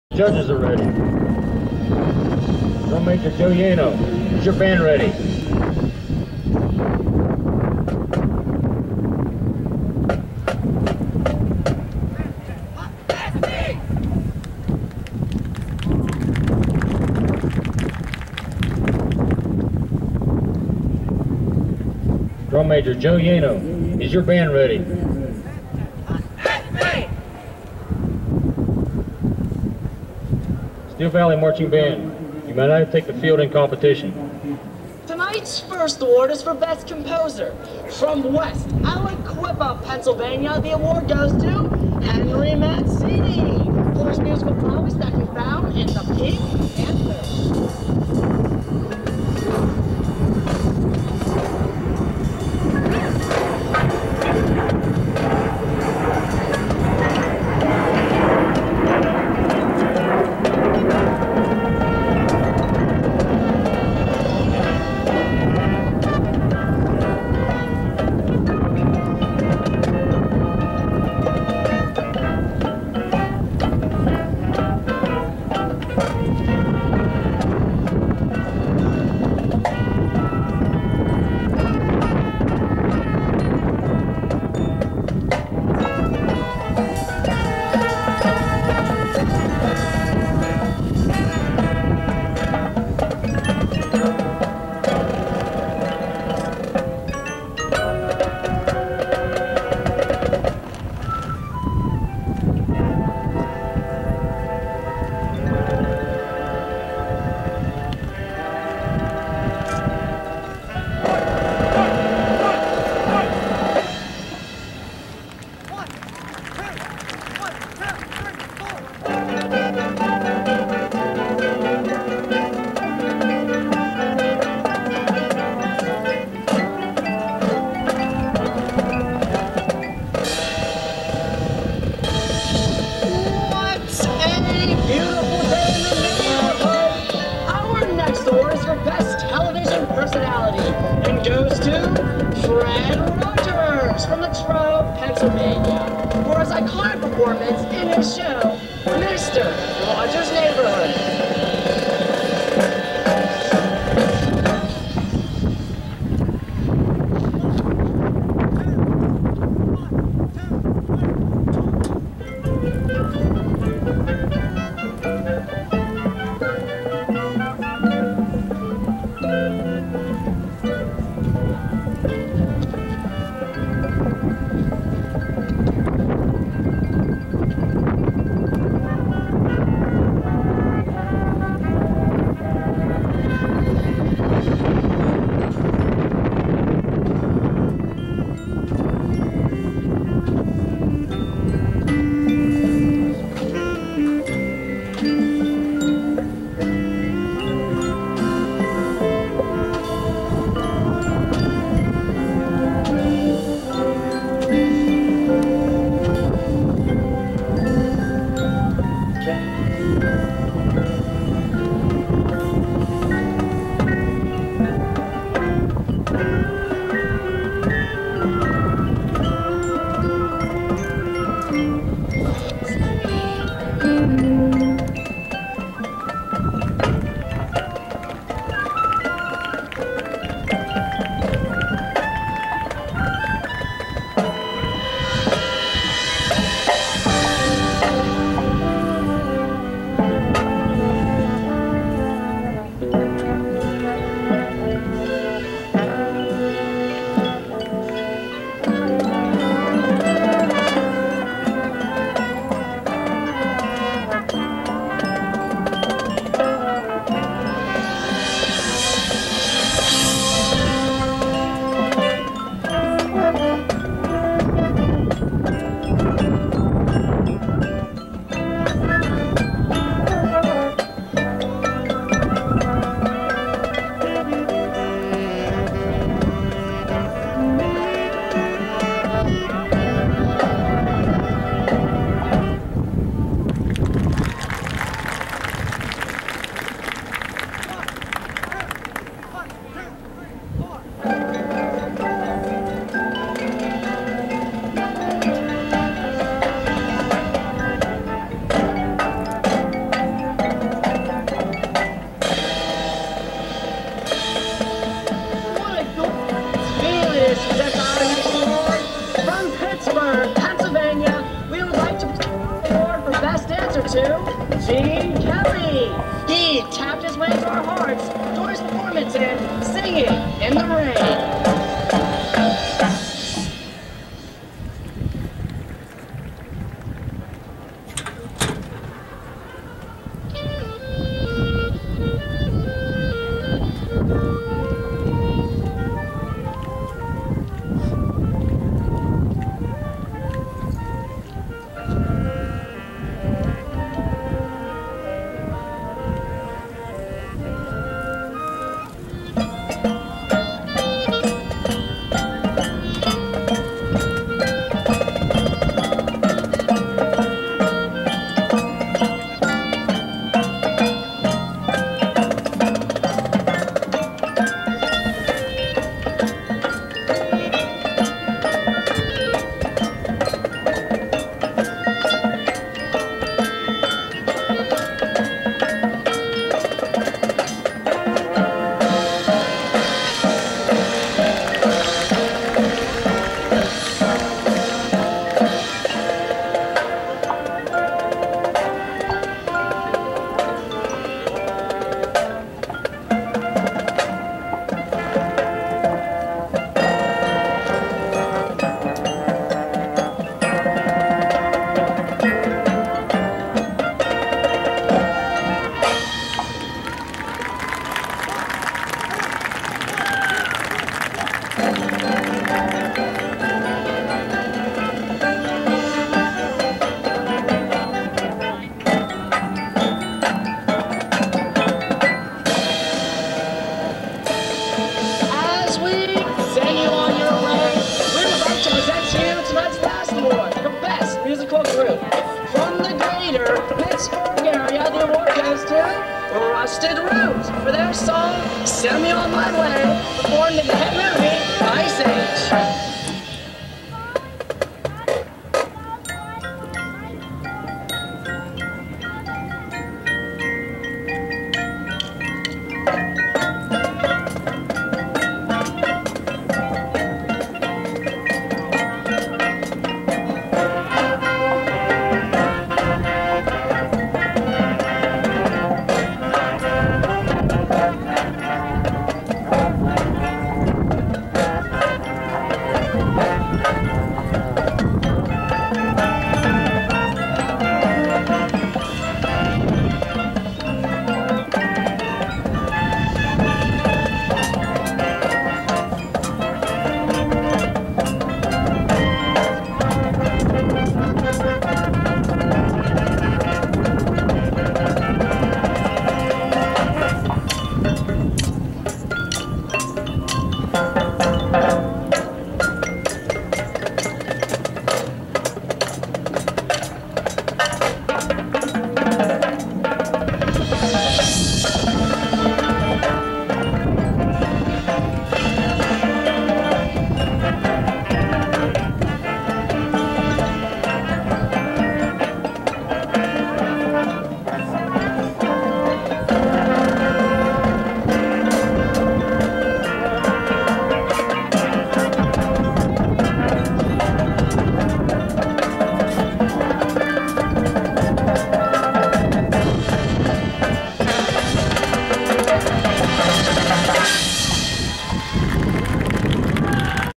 2012 Baldwin show
Scored 79.75 with High Aux & High percussion. I left the false start in.